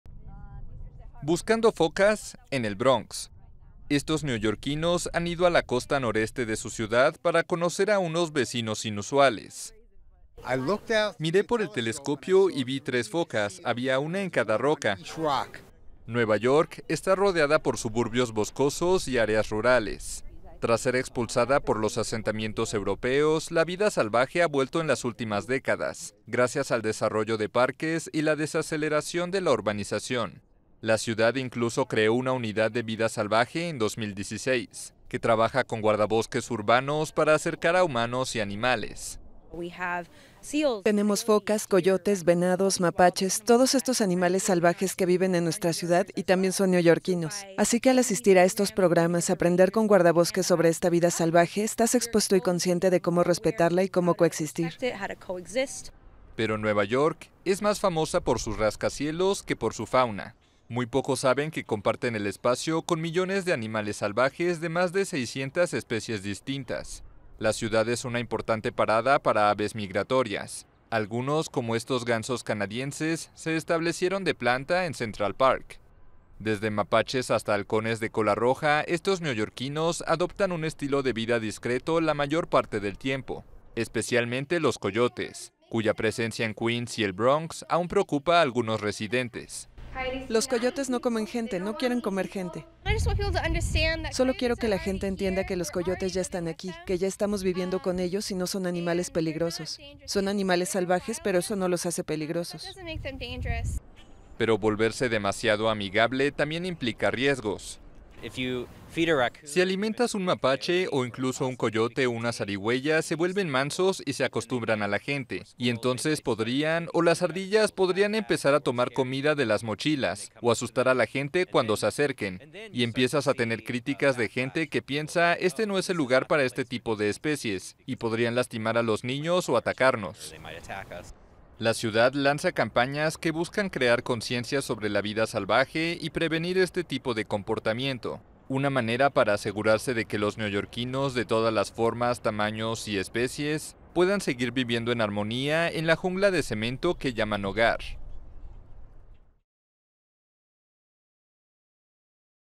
El verdadero lado salvaje de Nueva York
Coyotes en el Bronx, águilas calvas en Queens, mapaches y halcones de cola roja en Manhattan, búhos reales en Brooklyn o ciervos de cola blanca en Staten Island: una abundante fauna silvestre reside en Nueva York, la jungla de cemento.